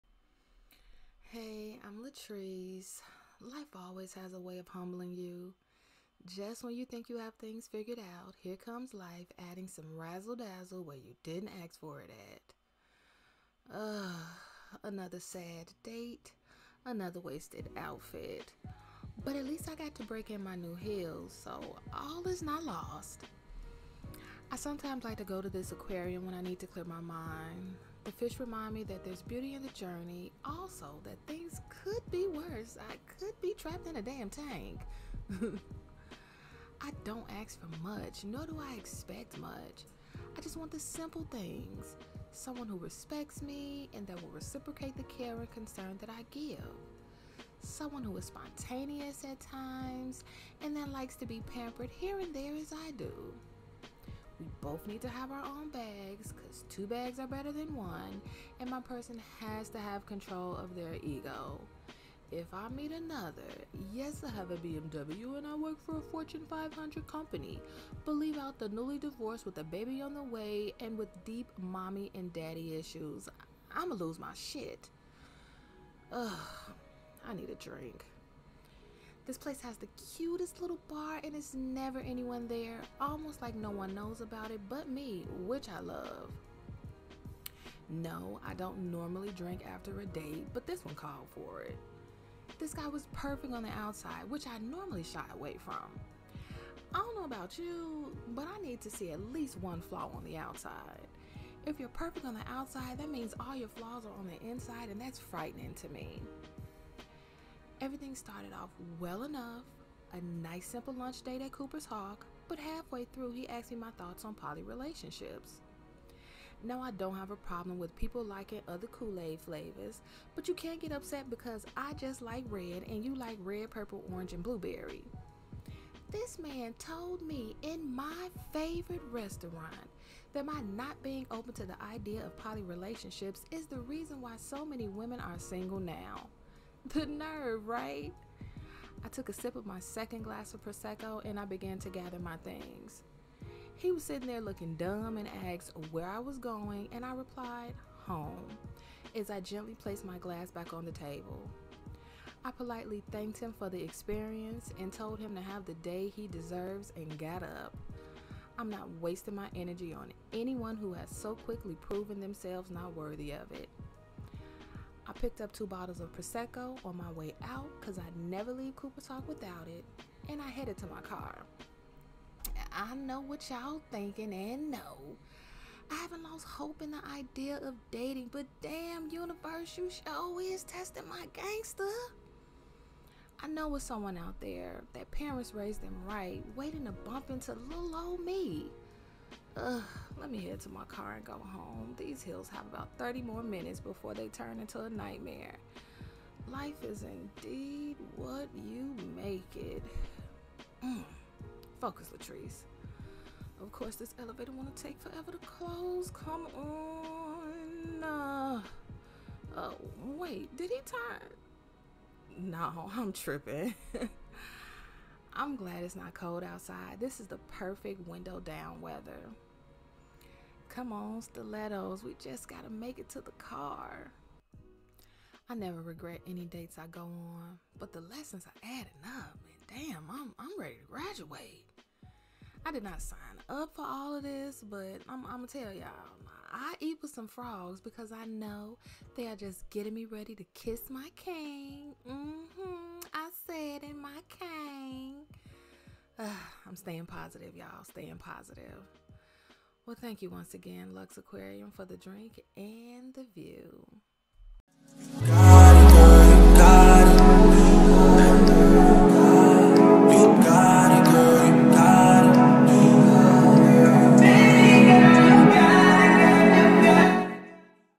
Storytime: Bad Date
Hey, y'all, In today's video, Storytime: Bad Date. I'll be going over the lousy date my fictional character "Latrice" had.  I'm trying something new and I hope you all enjoy it.